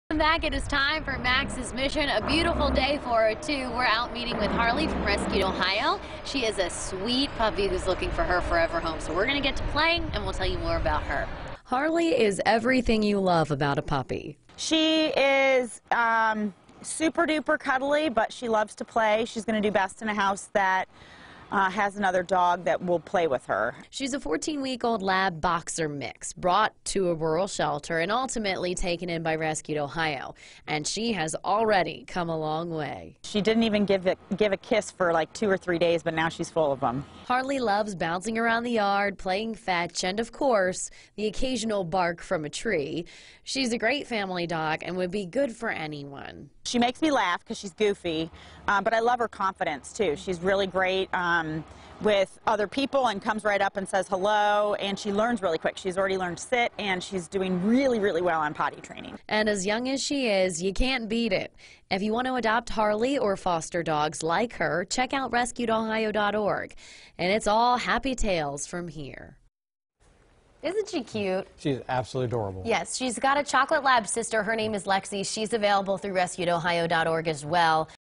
Max's Mission: Harley the lab/boxer mix needs a home
Harley loves bouncing around the yard, play fetch and of course.. the occasional bark from a tree.